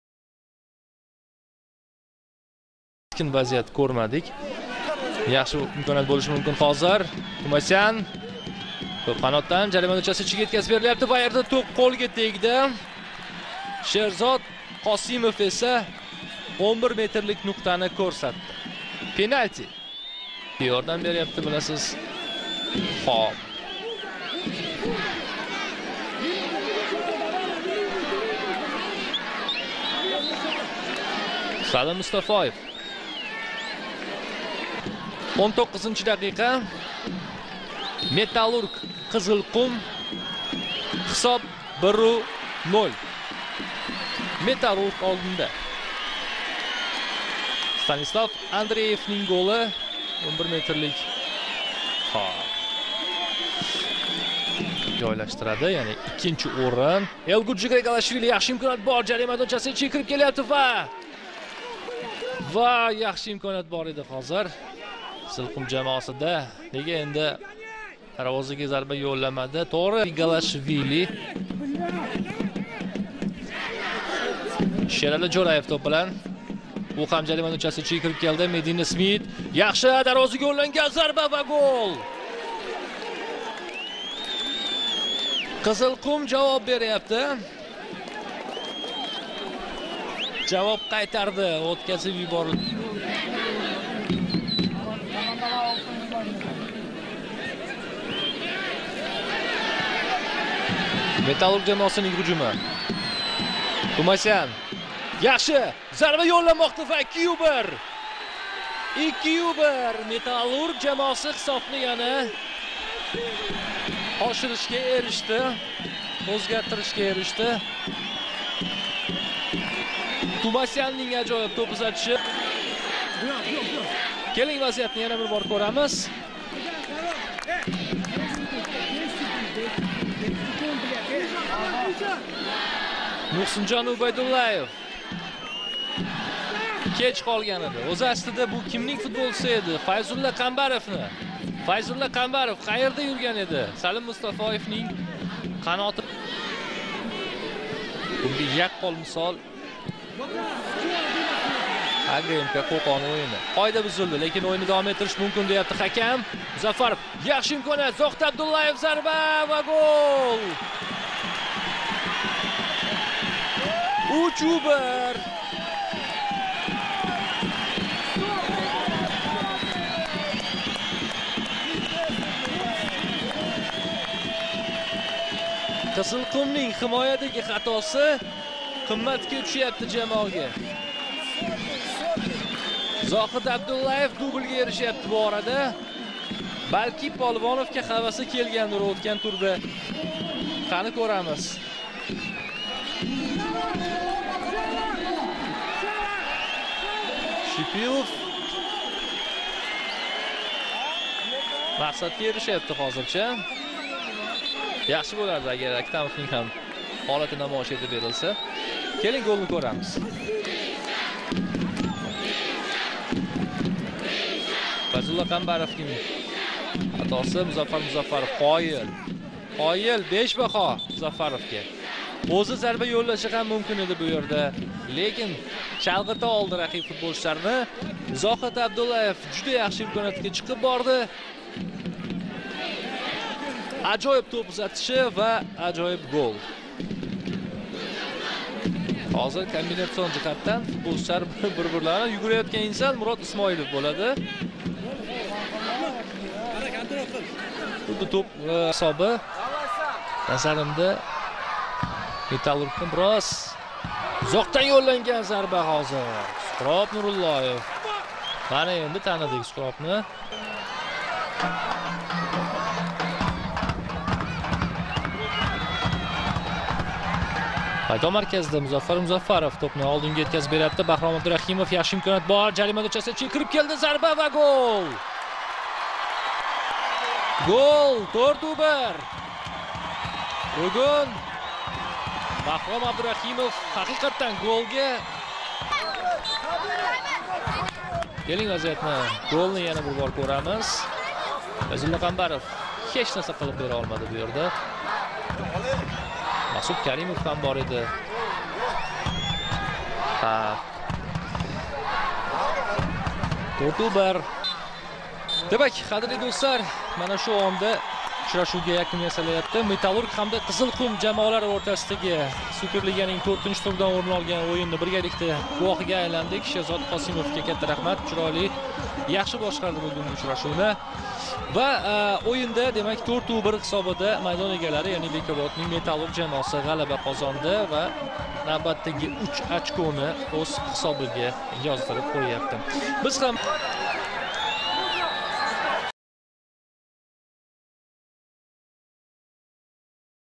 Superliga. Metallurg - Qizilqum 4:1. O'yin sharhi / CHAMPIONAT.ASIA
Superliga. Metallurg - Qizilqum 4:1.
Gollar: Stanislav Andreev 19, Zohid Abdullayev 35, 38, Bahrom Abdurahimov 90 - Mirjalol Qosimov 25.
O'yin sharhi / CHAMPIONAT.ASIA